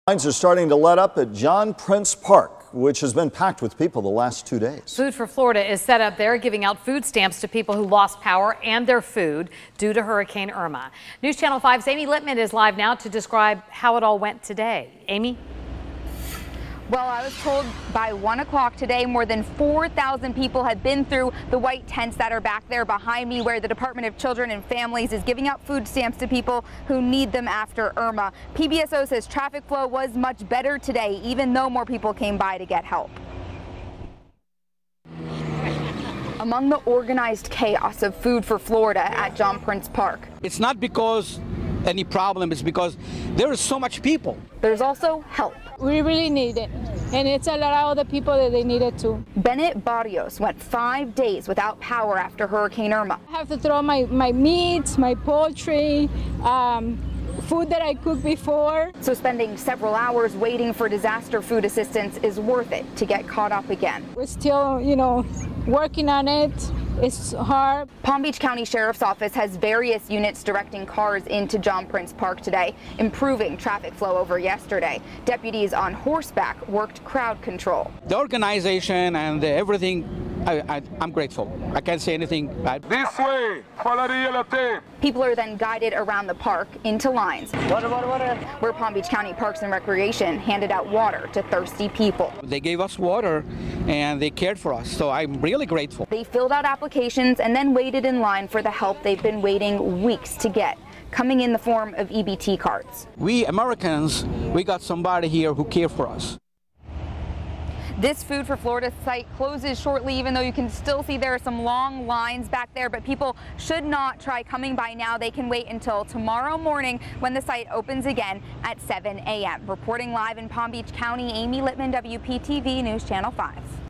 Long lines again on Day 2 of SNAP food assistance sign up sites in Palm Beach County
South Florida residents were once again eager Wednesday to get inside John Prince Park and Lake Ida Park for the second day of food relief assistance.